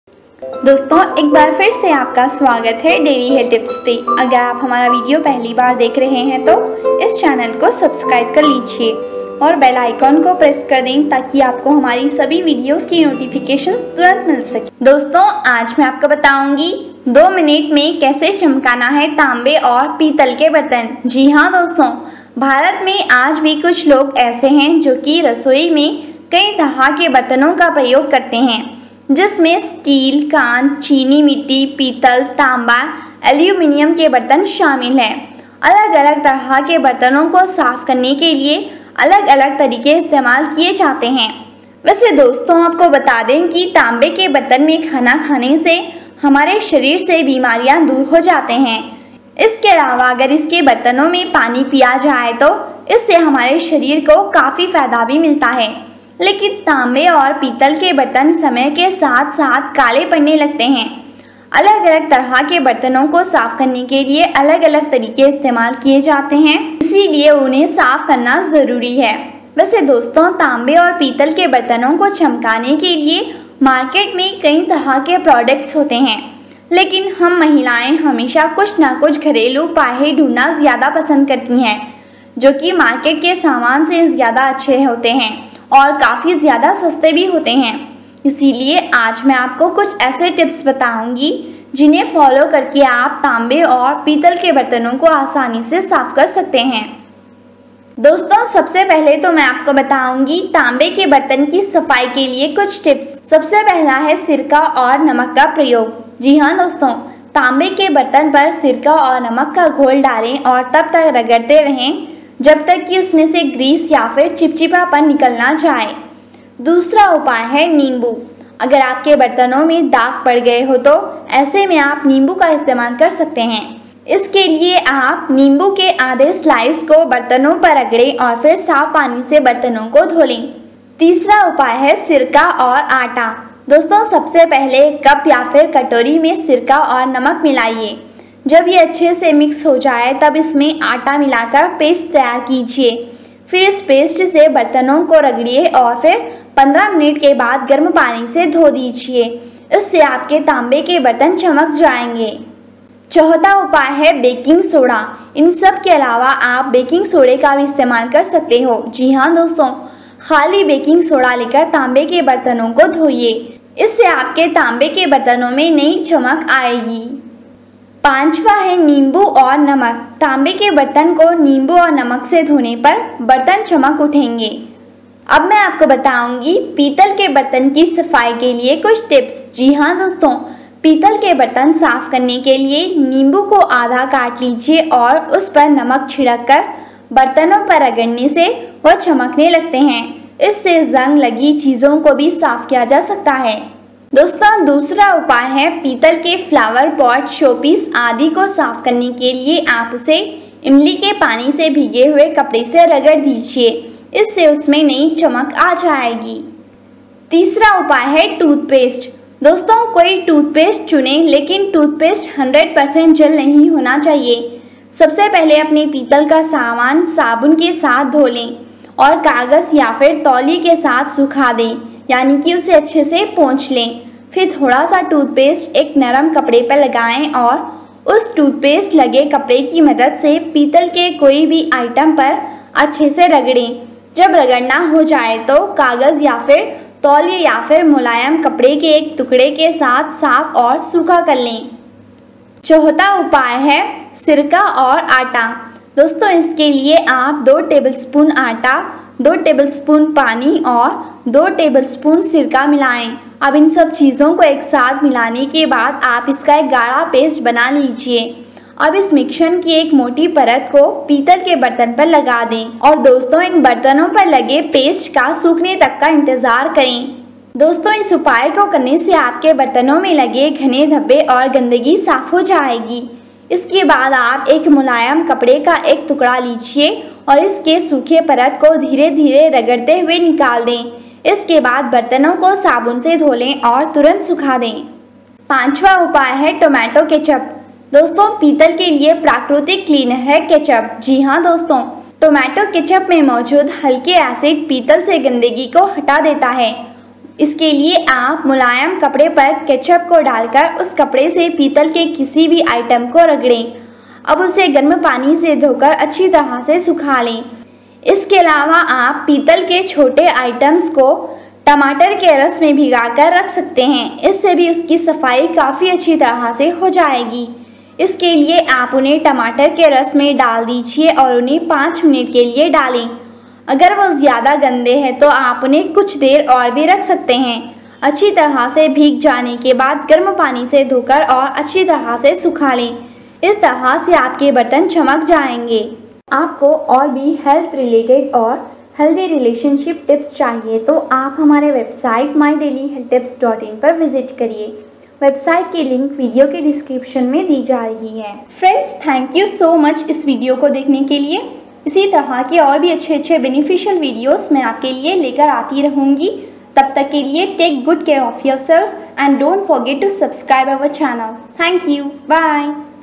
2 मिनट में चमकाएं तांबे और पीतल के बर्तन,तांबे और पीतल के बर्तन चमकाने का आसान घरेलु नुस्खा
2 मिनट में चमकाएं तांबे और पीतल के बर्तन,तांबे और पीतल के बर्तन चमकाने का आसान घरेलु नुस्खा.Read More Health Tips/Beauty Tips/Home Remedy @
भारत में आज भी कुछ लोग ऐसे हैं जो कि रसोई में कई तरह के बर्तनो का प्रयोग करते हैं जिसमें स्टील, कांच, चीनी-मिट्टी, पीतल, तांबा  ,एल्युमीनियम के बर्तन शामिल हैं। अलग-अलग तरह के बर्तनों को साफ करने के लिए अलग-अलग तरीके इस्तेमाल  किए जाते हैं।आपको  बतादें कि तांबे के बर्तन में खाना खाने से शरीर के रोग दूर हो जाते हैं।

इसके अलावा अगर इसके बर्तनों में पानी पिया जाए तो इससे शरीर को काफी फायदा भी मिलता हैं . लेकिन तांबे और पीतल , के बर्तन समय के साथ साथ काले पड़ने लगते हैं। अलग-अलग तरह के बर्तनों को साफ करने के लिए अलग-अलग तरीके इस्तेमाल  किए जाते हैं।  तांबे और पीतल  के बर्तन को चमकाने के लिये मार्केट में कई तरह के प्रोडक्ट्स होते है  हैं, लेकिन हम महिलाएं हमेशा कुछ ना कुछ घरेलू उपाय ही ढूंढना ज्‍यादा पसंद करती हैं, जो कि मार्केट के समान से ज्‍यादा अच्‍छे होते हैं।इसलिए  आज हम आपको कुछ ऐसे टिप्स बताने जा रहे है  जिनसे आप ताम्बे और पीतल के बर्तन आसानी से साफ़ कर सकते है